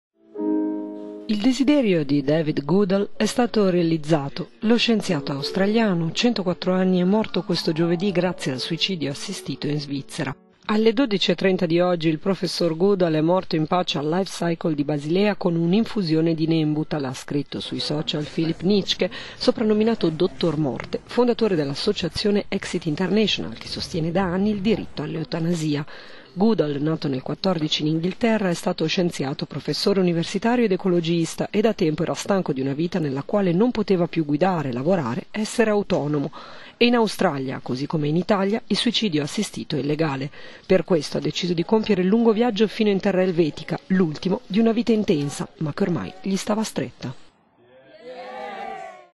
Suicidio assistito: lo scienziato australiano Goodall è morto in Svizzera
Se n'è andato con un'infusione di Nembutal, l'uomo di 104 anni che non era malato terminale, ma non voleva più vivere…